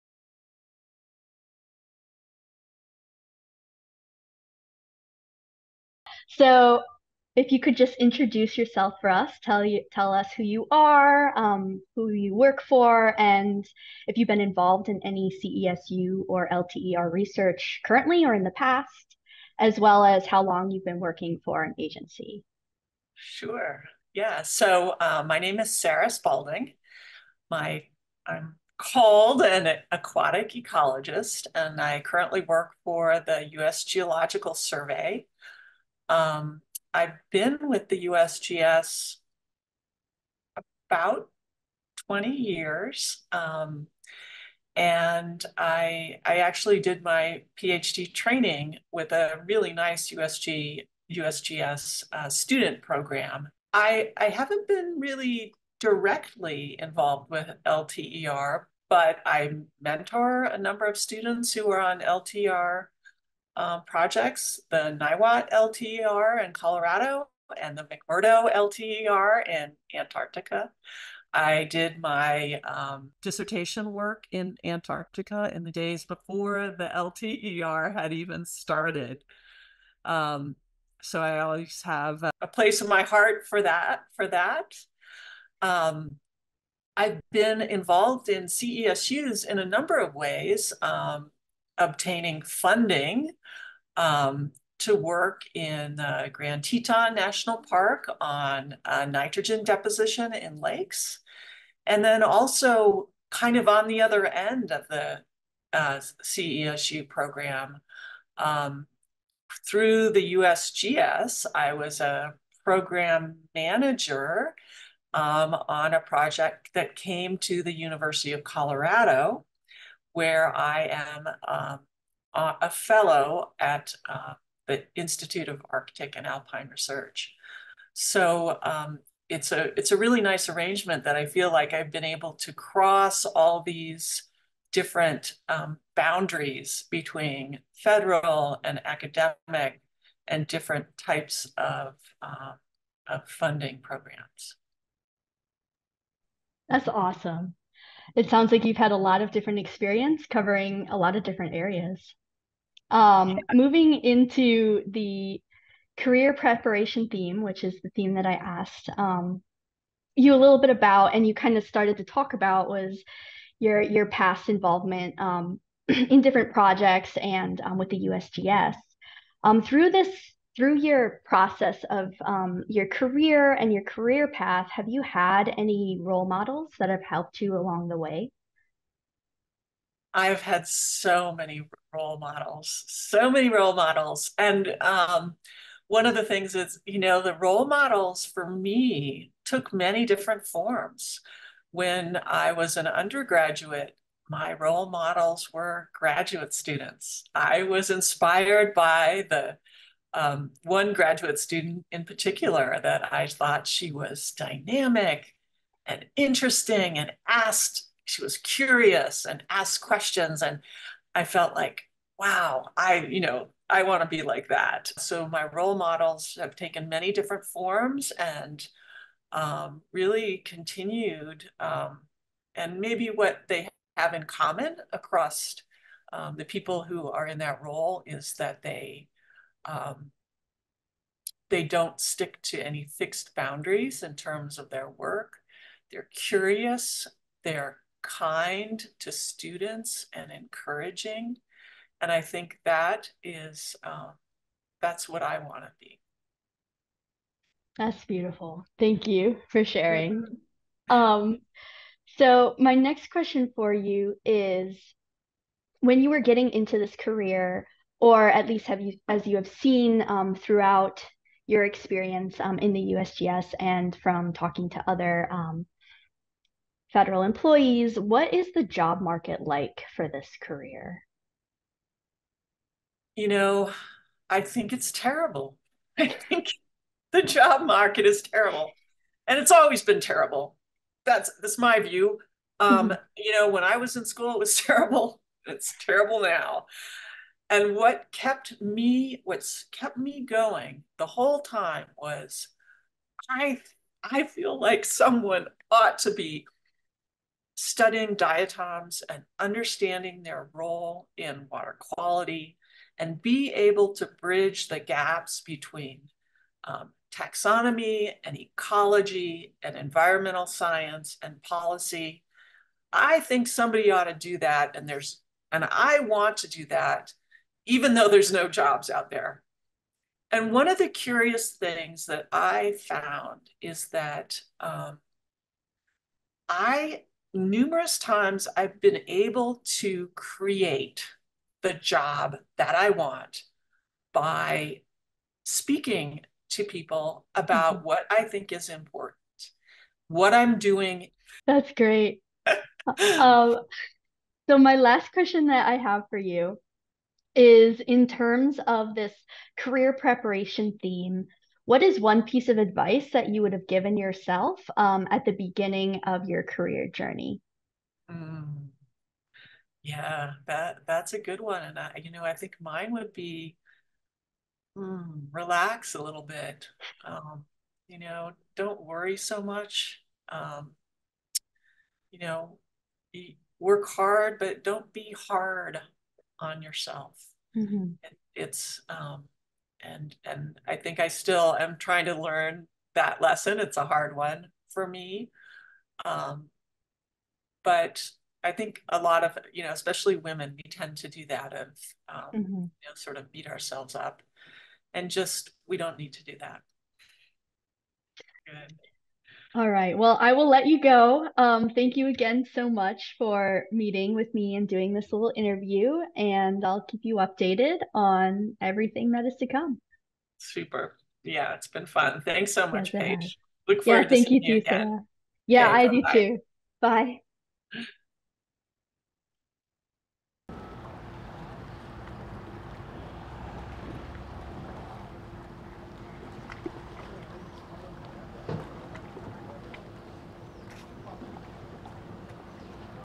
Sarah Spaulding | 2024 LTER-CESU Career Forum
Video interview with panelist Sarah Spaulding for the 2024 LTER-CESU Career Forum. Sarah is an aquatic ecologist with the USGS, and works with the McMurdo Dry Valleys LTER site.

For more information on the event, including registration for the upcoming panel discussion,

Bio: My professional interest lies in diatoms, which are single-celled organisms contained within silica cell walls.

I am interested in taxonomy, paleoecology, systematic, assessment and biogeography of diatoms. My research is directed toward understanding diatom biology and the ecological roles of diatoms on the planet. I am passionate about sharing diatom taxonomy and ecology with scientists, students and the public.